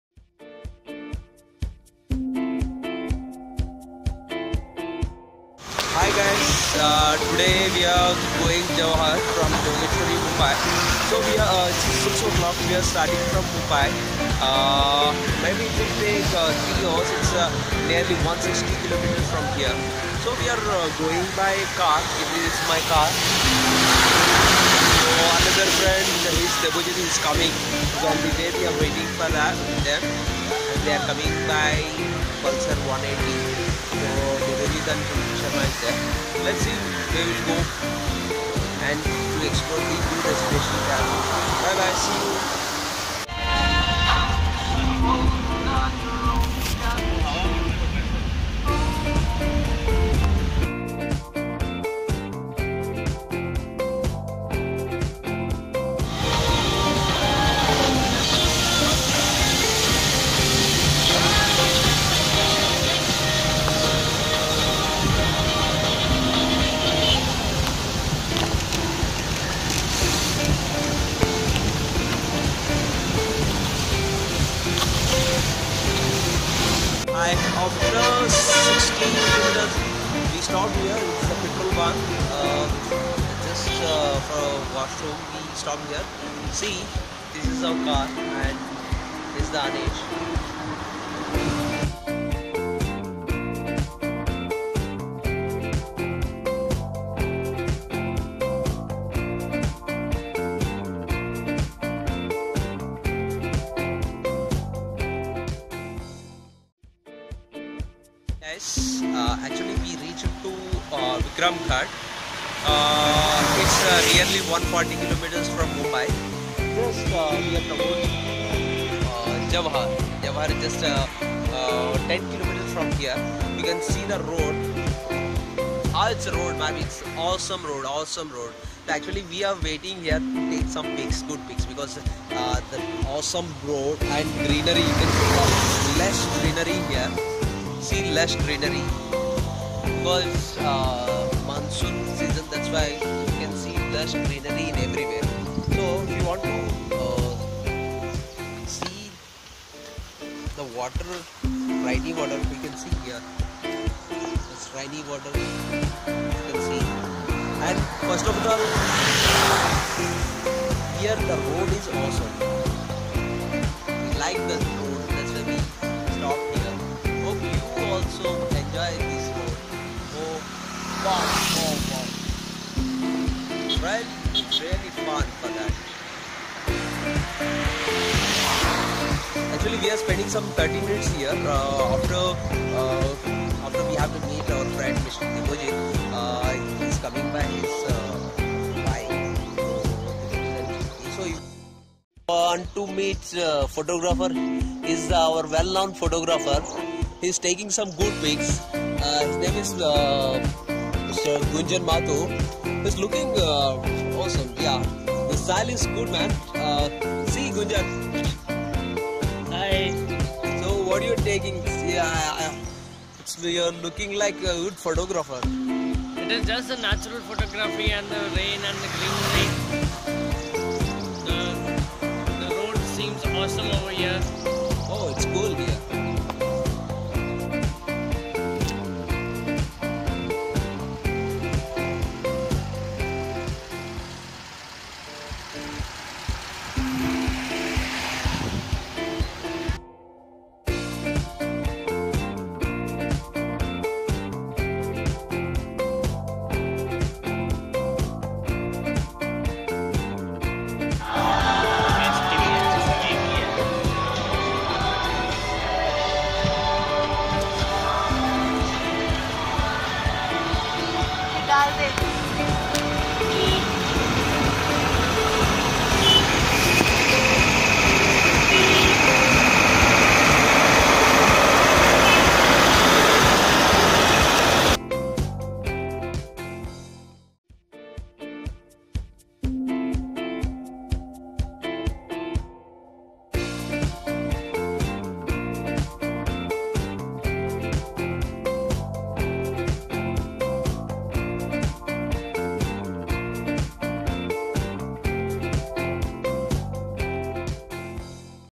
Jawhar Trip from Mumbai
We group of friends planned to visit near destination from Mumbai,jawhar it's 150 km far from Mumbai and this place best visit on monsoon season